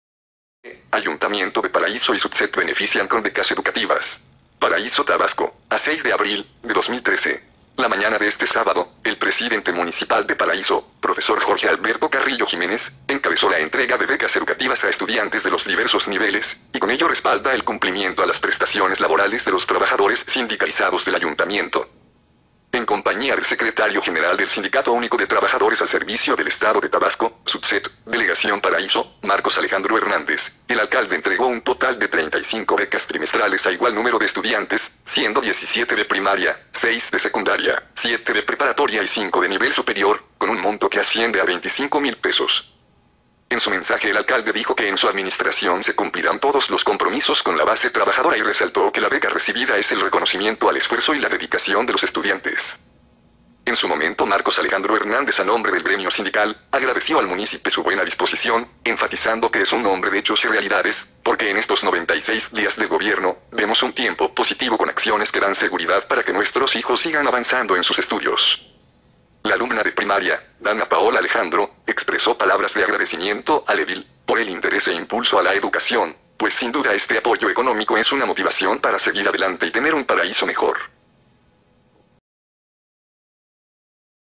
2013-04-06 Ayuntamiento de Paraíso y SUTSET benefician con becas educativas
Ayuntamiento de Paraíso y SUTSET benefician con becas educativas.
Paraíso Tabasco, a 6 de abril de 2013.
La mañana de este sábado, el Presidente Municipal de Paraíso, Profesor Jorge Alberto Carrillo Jiménez, encabezó la entrega de becas educativas a estudiantes de los diversos niveles; y con ello respalda el  cumplimiento a las prestaciones laborales  de los  trabajadores sindicalizados del ayuntamiento.

En compañía del Secretario General del Sindicato Único de Trabajadores al Servicio del Estado de Tabasco (SUTSET), Delegación Paraíso, Marcos Alejandro Hernández; el alcalde entregó un total de 35 becas  trimestrales a igual número de estudiantes, siendo 17 de primaria, 6 de secundaria, 7 de preparatoria y 5 de nivel superior; con un monto que asciende a 25 mil pesos.

En su mensaje el alcalde dijo que en su administración se cumplirán todos los compromisos con la base trabajadora y resaltó que la beca recibida es el reconocimiento al esfuerzo y la dedicación de los estudiantes.

En su momento Marcos Alejandro Hernández a nombre del gremio sindical, agradeció al munícipe su buena disposición; enfatizando  que "es un hombre de hechos y realidades, porque en estos 96 días de  gobierno, vemos un tiempo  positivo con acciones que dan seguridad para que nuestros hijos sigan avanzando en sus estudios".

La alumna de Primaria, Dana Paola Alejandro, expresó palabras de agradecimiento al edil, por el interés e impulso a la educación, pues sin duda este apoyo económico es una motivación para seguir adelante y tener un Paraíso  mejor.